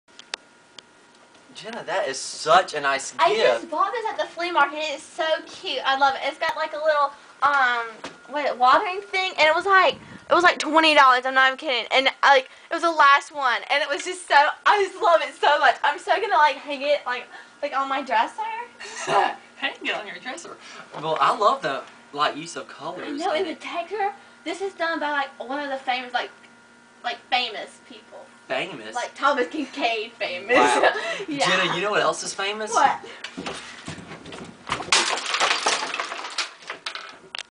Cousin Video: The Horse
The results of when favorite cousin lucas shows favorite cousin jenna the ring.